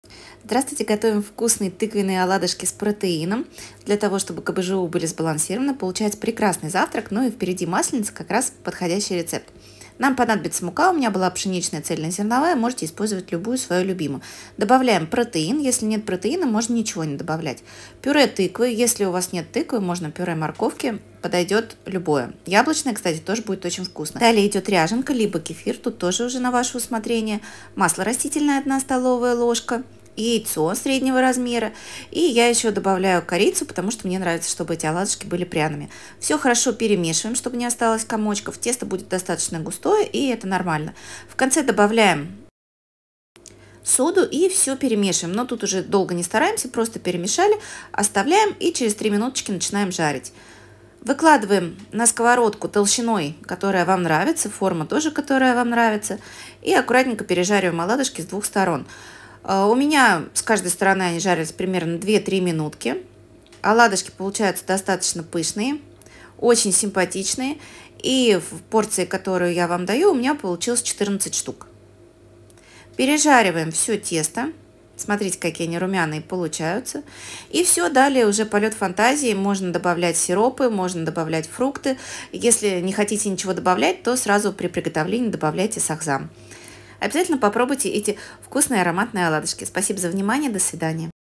ПП протеиновые оладушки с тыквой, много белка, полезный завтрак, пп блинчики
Поддержка канала:
Тинькофф: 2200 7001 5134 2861

Ингредиенты:
Ряженка 2.5% - 80 г ( можно кефир) комнатной температуры
Яйцо - 1 шт ( с1 )
Мука - 50 г ( у меня пшеничная цз)
Тыквенное пюре - 100 г ( у меня детское), можно морковное или яблочное
Протеин - 30 г по желанию ( КБЖУ с протеином)
Сода - 0.3 чл
Корица - по желанию

КБЖУ на 100 г 190 ккал 11.8/ 7.9/ 16.5